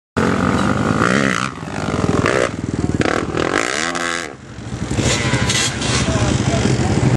07 suzuki lt450 jumps wall
07 suzuki 450 flys up wall at pitts